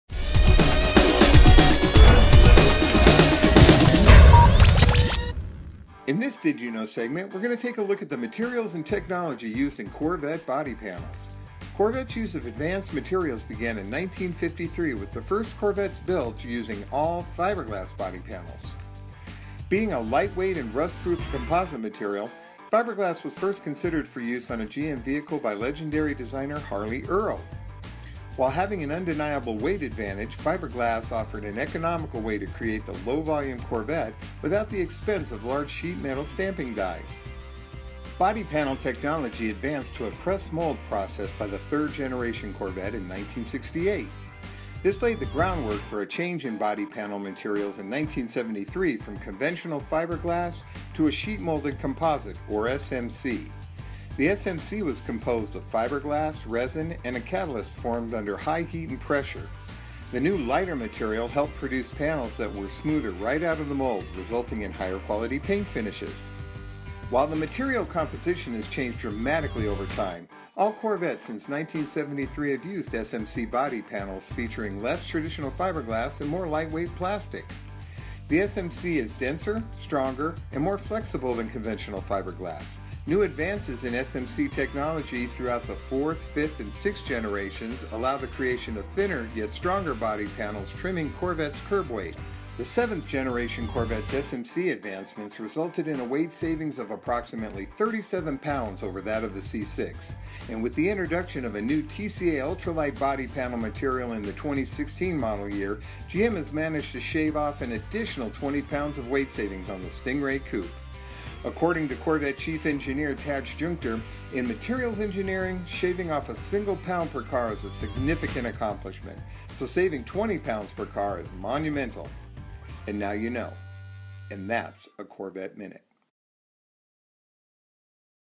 Corvette Minute - Body Panel Material and Technology
In this Corvette Minute we're talking about Corvette body panel materials and technology.  From the original fiberglass panels to modern composite materials, Corvette has been an industry leader in light weight body panels.